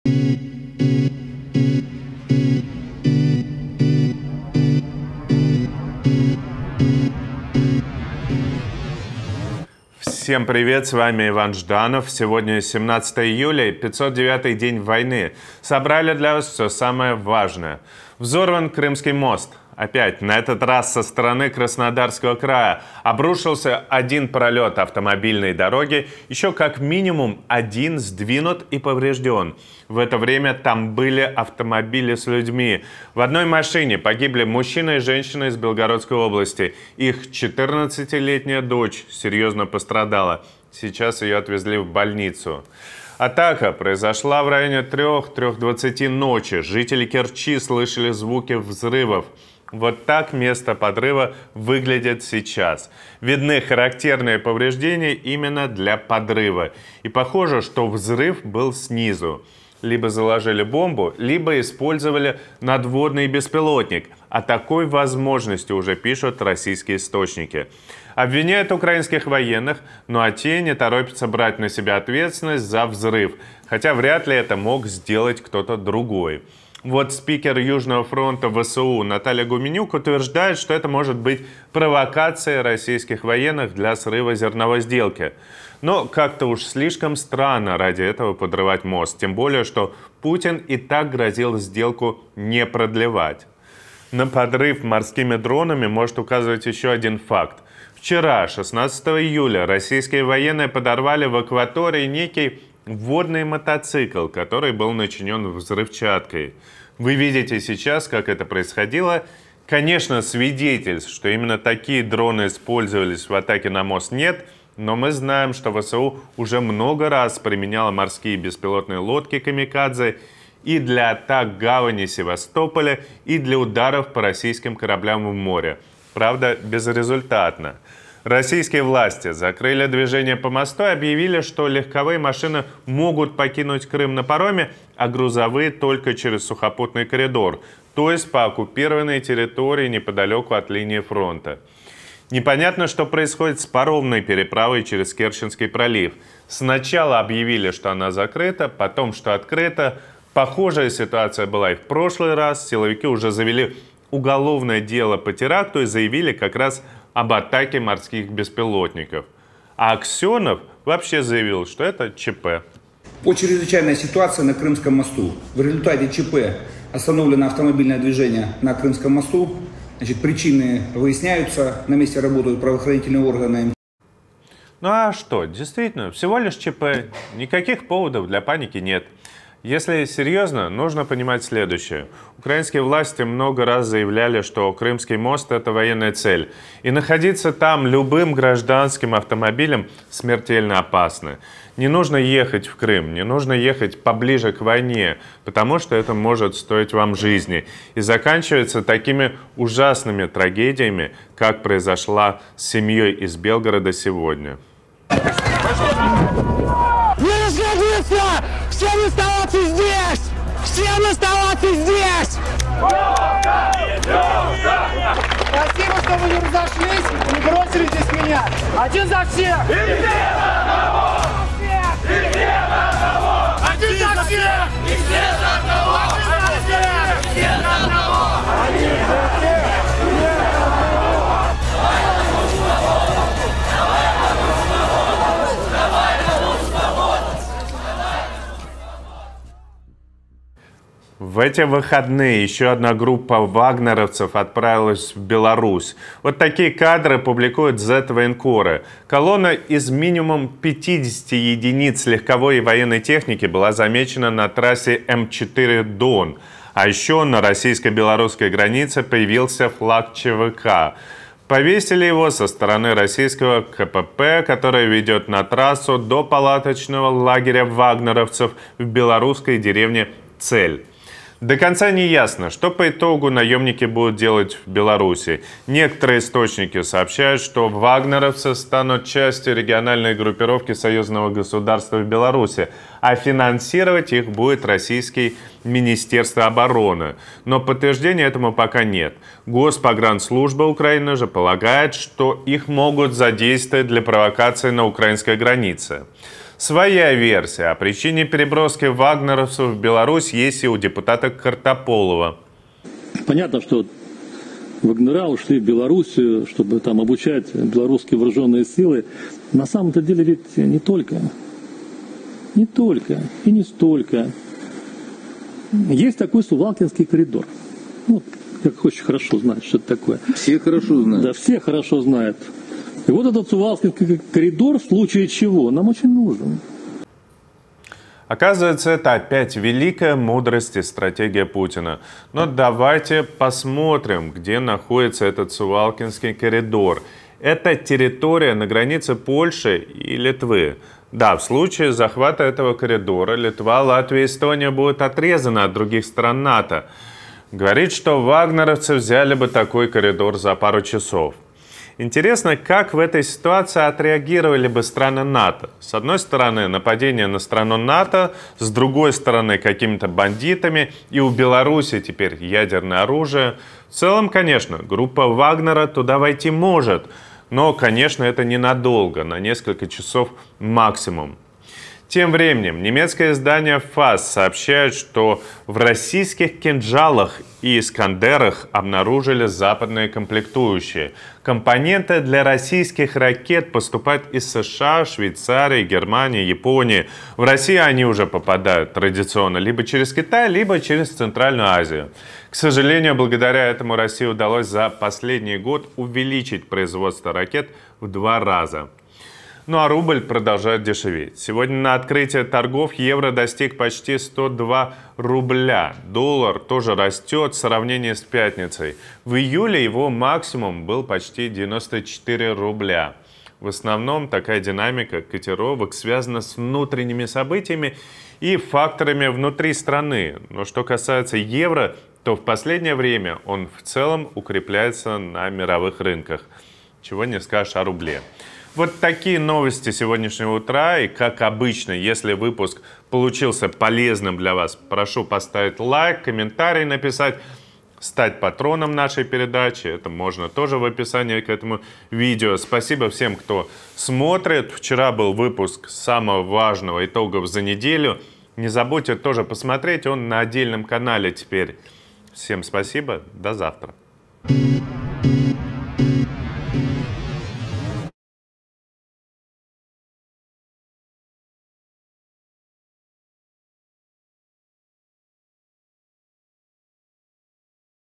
❗️ НОВОСТИ | КРЫМСКИЙ МОСТ ВЗОРВАН. ВСЕ ПОДРОБНОСТИ
509 день войны. Снова взорван Крымский мост, погибли двое. Ещё одна группа вагнеровцев отправилась в Беларусь. В российских ракетах «Кинжал» и «Искандер» обнаружили западные комплектующие. А рубль продолжает дешеветь.

🔻 Bitcoin
3QzYvaRFY6bakFBW4YBRrzmwzTnfZcaA6E
🔻 Ethereum
0x314aC71aEB2feC4D60Cc50Eb46e64980a27F2680 "
🔻 Monero
42e1hkdsTHUUWSM24jVgLTH4MDJPMbNS1XDt7rK36dTBSKweohz9FSWKAAoqHLN9nSVgocnSnkR2AaLMWtsrmAoQGPdWSE1
🔻 Zcash (Z-адрес)
zs1x0f6ccg9njmkahy03kup0fujdgp5pthv2nug7mwe0luj54mxal60ttym4aex3fnmj900yqtqcl5
🔻 USDT
0x9A4B20a7909Af03dd8B4f28A419840F9715E1F73